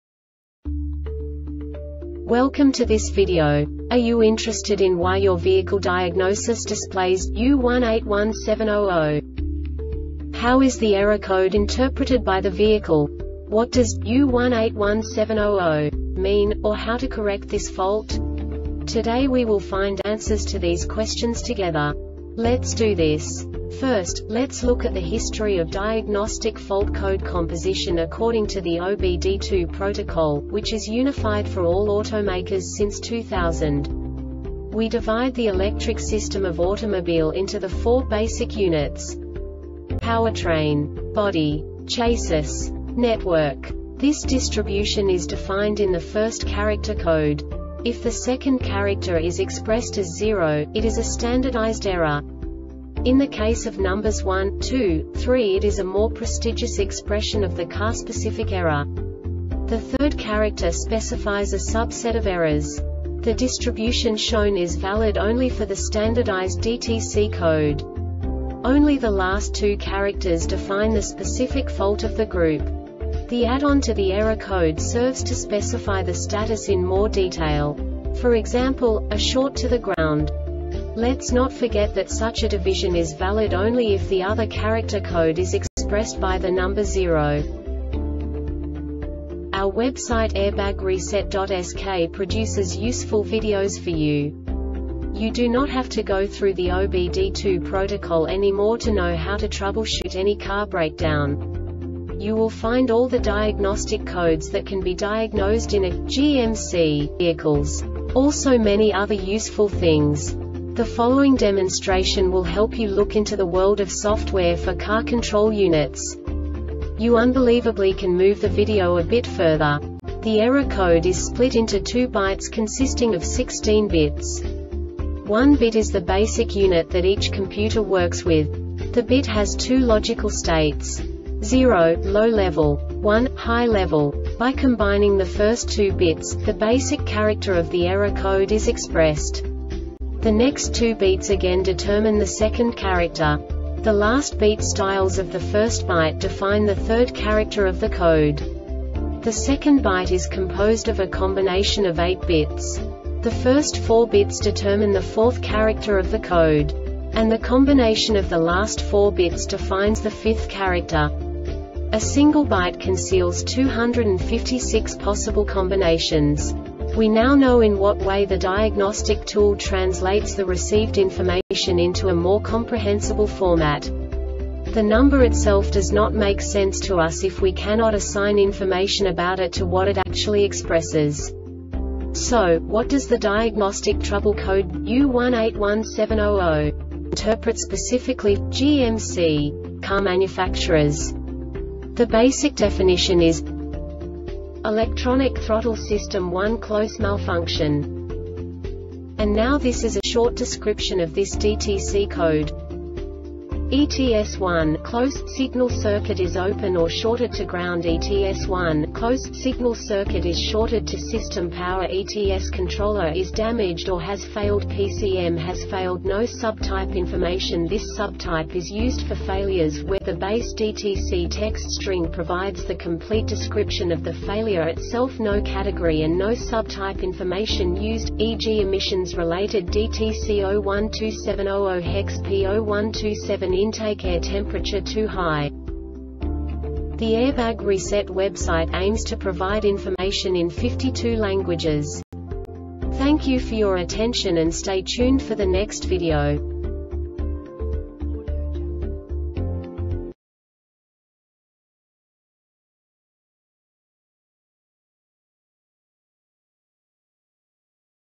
DTC GMC U1817-00 Short Explanation
Contents:
0:21 Basic DTC analysis according to OBD2 protocol standard.
1:48 Insight into programming
2:58 A diagnostically understandable description of the Diagnostic Trouble Code
3:05 Basic error definition

Make
GMC
Code
U1817 00
Definition
Electronic Throttle System 1 Close Malfunction
Cause
ETS 1 "close" signal circuit is open or shorted to groundETS 1 "close" signal circuit is shorted to system powerETS controller is damaged or has failedPCM has failed
Failure Type